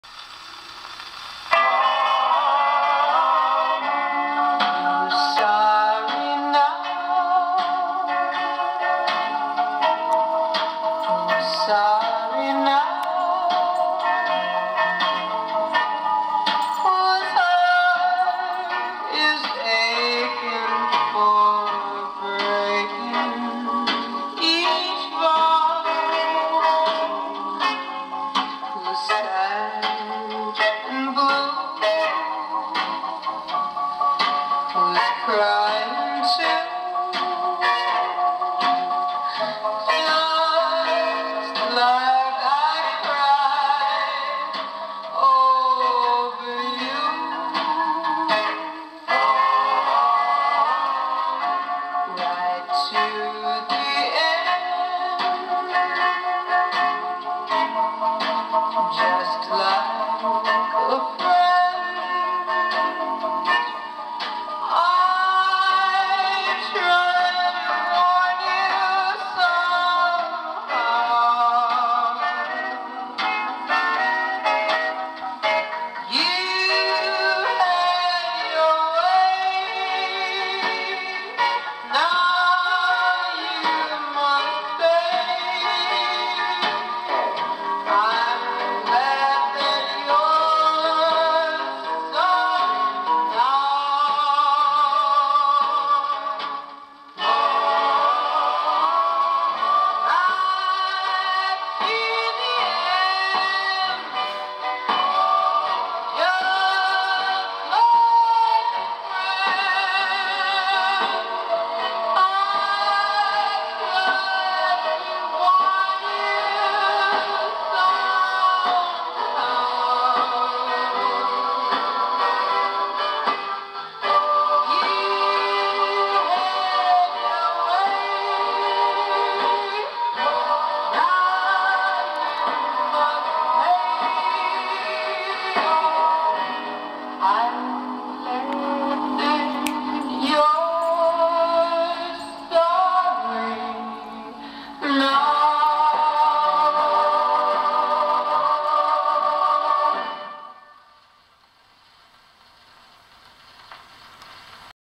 Hector, gramophone and Connie Francis
Our Cocker Spaniel listening to HMV 102 wind up gramophone